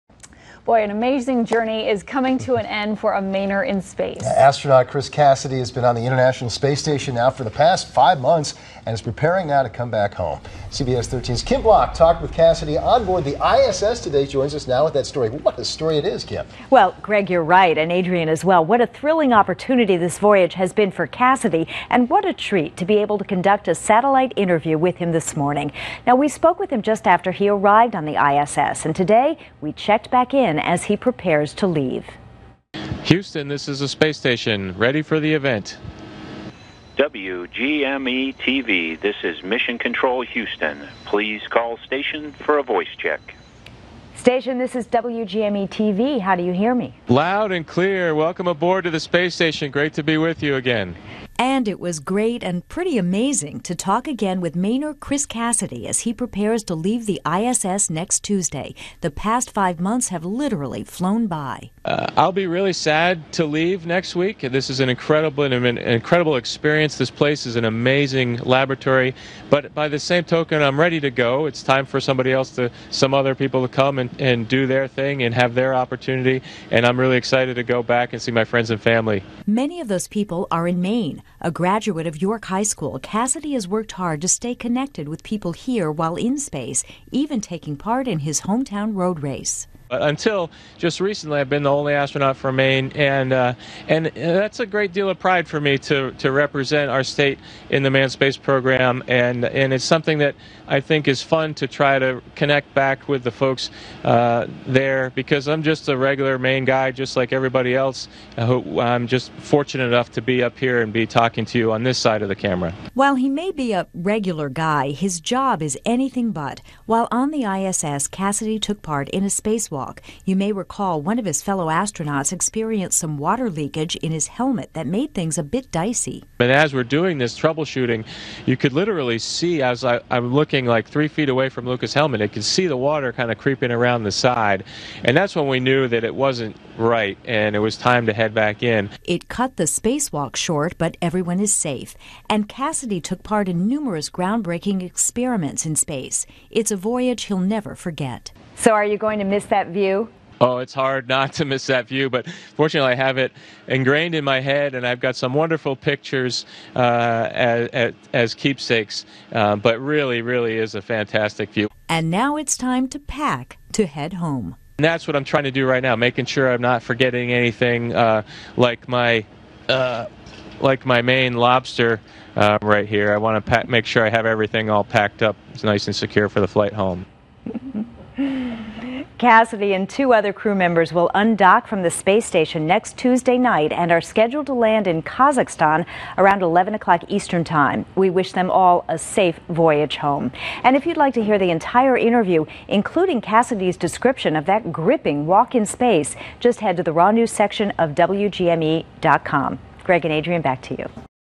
Interview with astronaut Chris Cassidy
INTERNATIONAL SPACE STATION (WGME) -- Astronaut Chris Cassidy, from York, is preparing to leave the International Space Station after a five month mission.In our exclusive satellite interview, Cassidy says the time has literally flown on board the I.S.S., where he has taken part in numerous experiments and a spacewalk to make some repairs on the station.Cassidy and two Russian cosmonauts are scheduled to leave the I.S.S. next Tuesday night, bound for Kazakhstan.   He is preparing now for re-entry.Cassidy says he'll most definitely miss the view, but is ready to see friends and family in Maine.